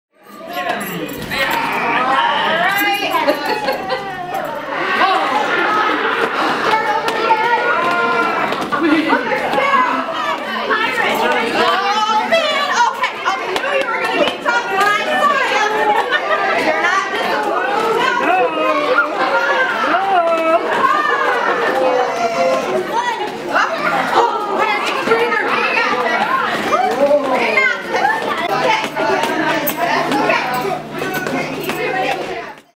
Sparring with Sparrow
Children went up against pirates during Sparring with Sparrow at Loutit District Library. The Michigan Pirate Festival continues with events throughout the week.